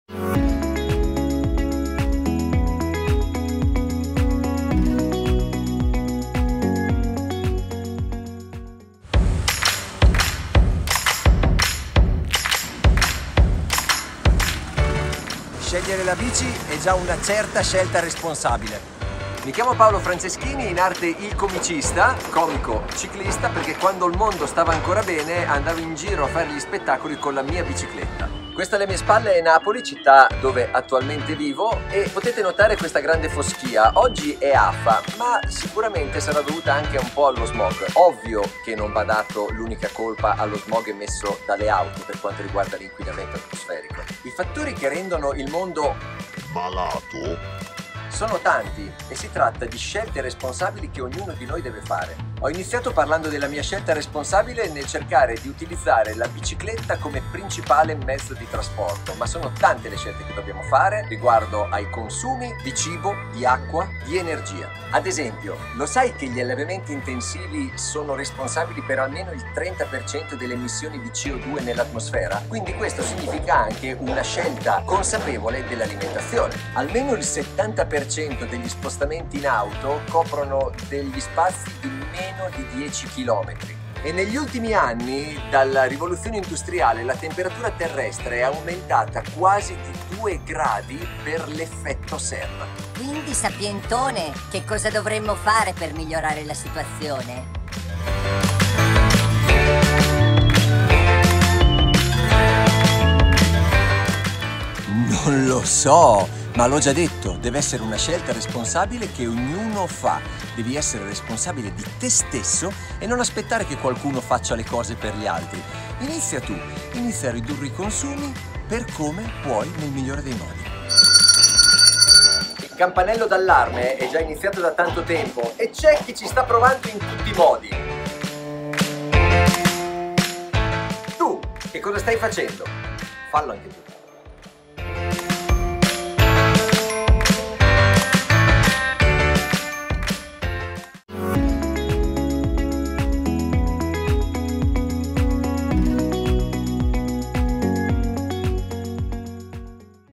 Paolo Franceschini - videopillole - ISOLA URSA SAIE 2021
Paolo Franceschini - Conduttore radiofonico e "Comicista" (comico - ciclista)
per ISOLA URSA a SAIE Bari 2021